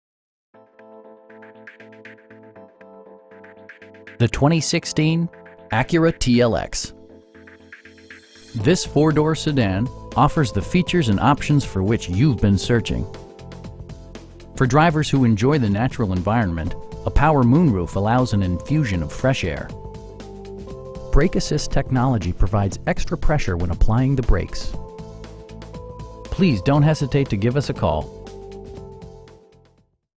2016 Acura TLX 3.5 V-6 9-AT P-AWS in Ocala, FL 34471
Jenkins Acura
2025 S.W. College Road

The 2016 Acura TLX. This 4 door sedan offers the features and options for which you've been searching. For drivers who enjoy the natural environment, a power moon roof allows an infusion of fresh air. Brake assist technology provides extra pressure when applying the brakes. We have the vehicle you've been searching for at a price you can afford. Please don't hesitate to give us a call.